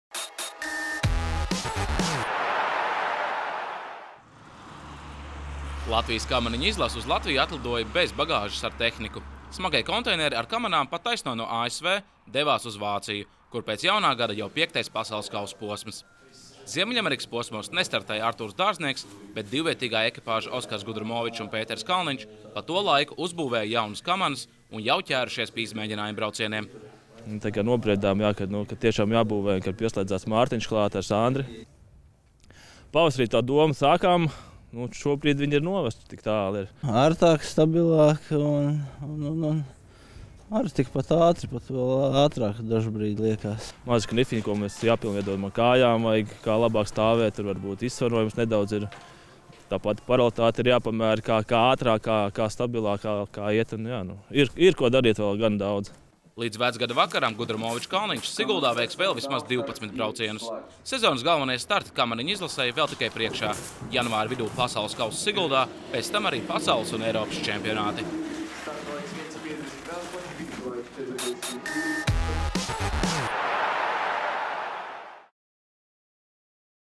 Kamaniņu sporta izlase gatavojas nākamajiem PK posmiem
Latvijas kamaniņu sporta izlases dalībnieki Oskars Gudramovičs/Pēteris Kalniņš izlaida trīs Ziemeļamerikas posmus. Tā vietā vīri izmēģināja jaunās kamaniņas Vinterbergā un Siguldā.